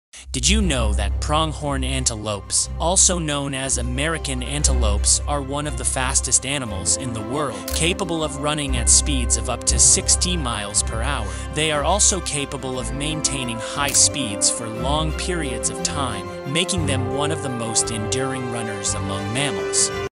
Fastest Pronghorn Antelopes
Did you know that Pronghorn antelopes, also known as American antelopes are one of the fastest animals in the world? Capable of running at speeds of up to 60 miles per hour. They are also capable of maintaining high speeds for long periods of time, making them one of the most enduring runners among mammals.